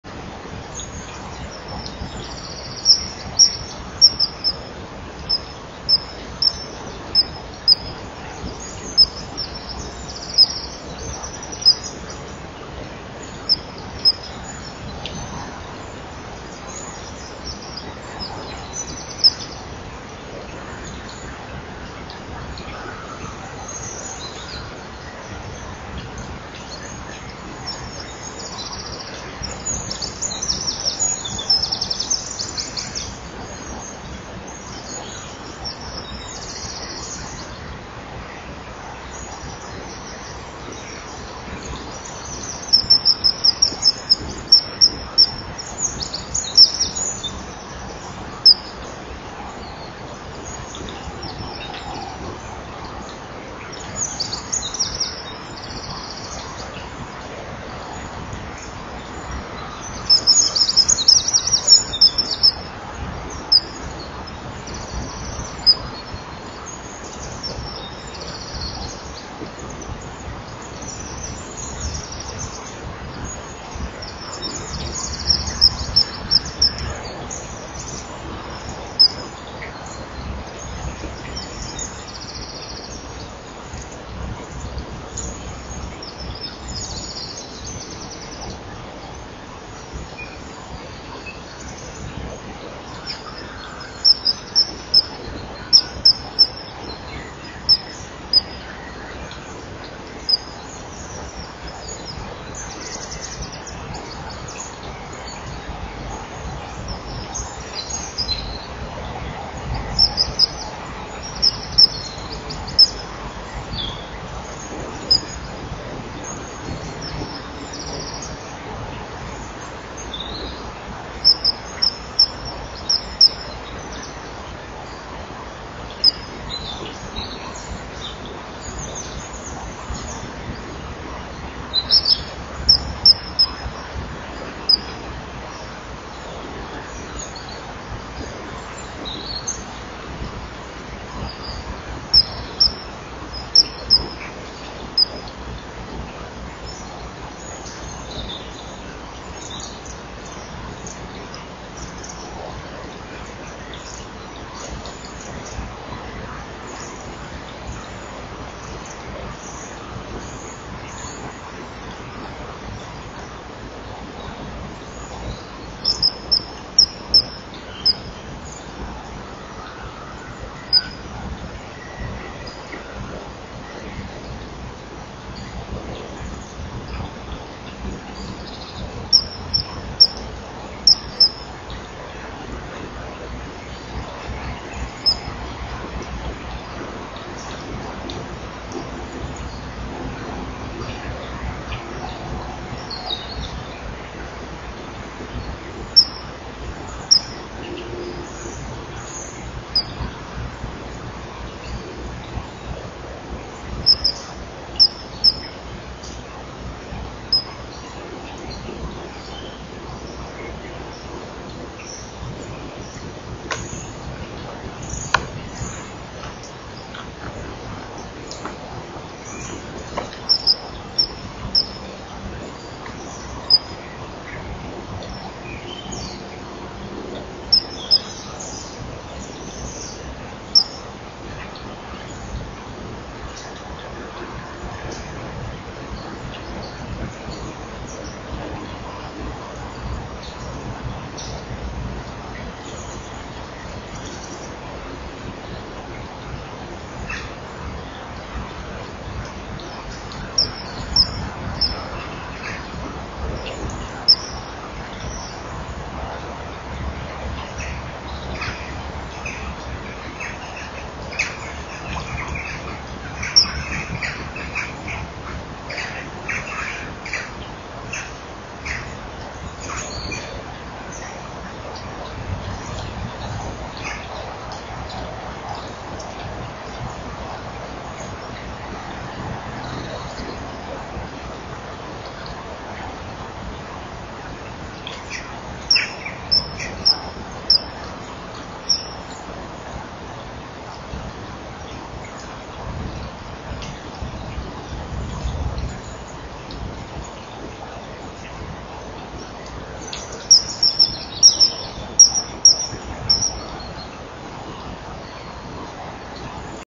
broedwandcam 2012 10 18 105453 IJ onderzoek veel gangen
10.54 uur. IJsvogel vliegt achter op de stok en onderzoekt veel gangen, waaronder een aantal keer het linkerkunstnest, waar hij zelfs even een beetje de gang in gaat. Nog niet helemaal naar binnen, maar hij heeft wel gekeken. Het leek of er nog een in de buurt was, maar die hebben we niet gezien.